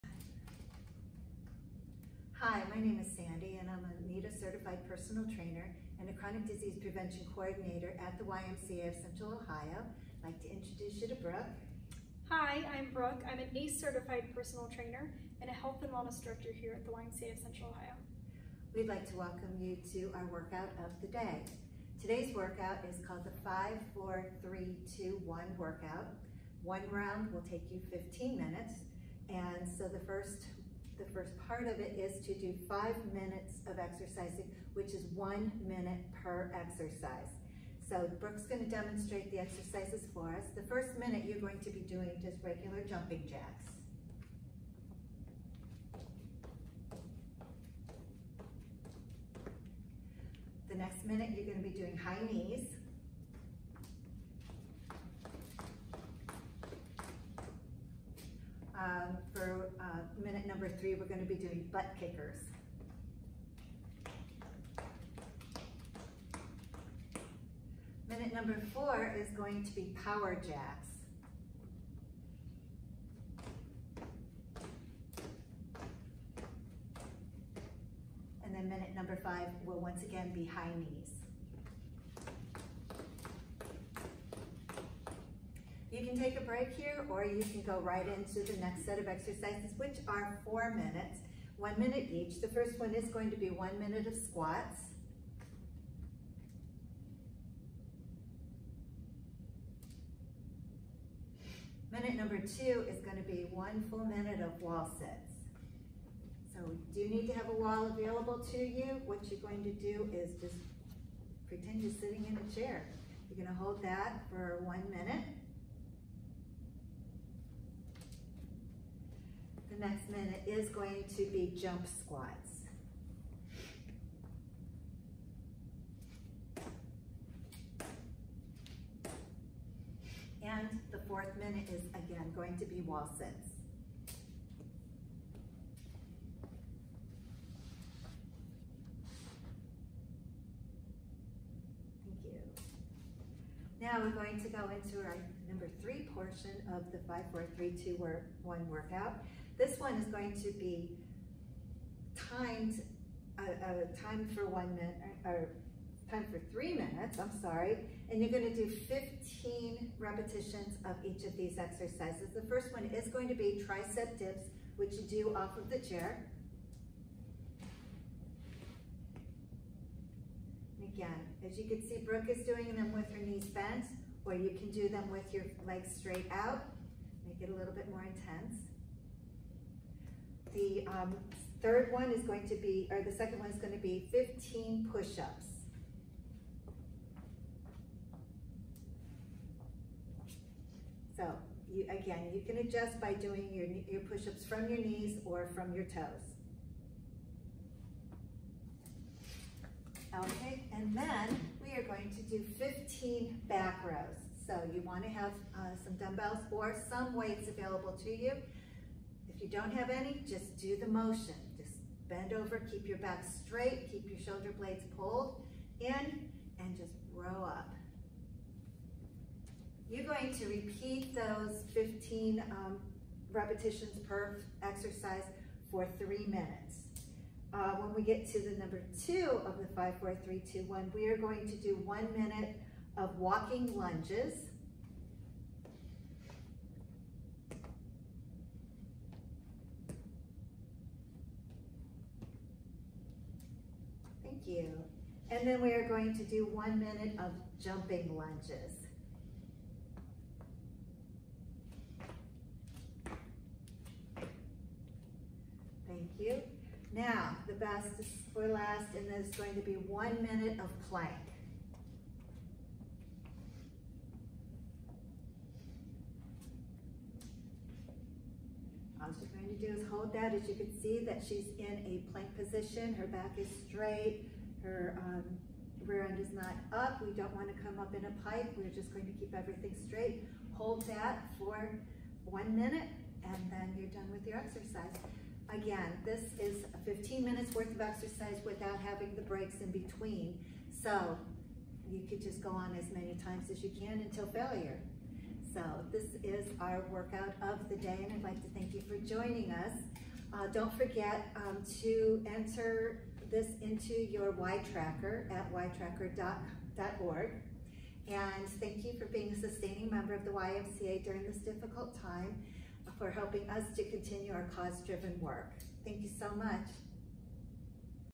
Web WOD - Pure Strength | #1
Web WOD | Thursday, March 26, 2020

Pure Strength | #1
Author: Hannah Jo, Gahanna/John E. Bickley YMCA

Equipment: Dumbbells, mat optional
Estimated Time to Complete: 45-60 minutes

The first set of the workout is intended to be high reps with low weight. The second set of the workout is intended to be high weight with low reps. Feel the burn!

Warm-up | 60 seconds each
Squats
Lunges, alternating
Curtsy lunge with lat pull down, alternating
Slow motion burpee

Work set #1 | 60 seconds each, AMRAP (as many reps as possible) with low weight
Dumbbell squat
Renegade row
Deadlift
Weighted tricep dip
Dumbbell overhead alternating lunges
Front raise/lateral raise combo
Bicep curl to overhead press
Weighted russian twist
Complete 2 or 3 times

Work set #2 | 10 reps each with higher weight
Dumbbell squat
Renegade row
Deadlift
Weighted tricep dip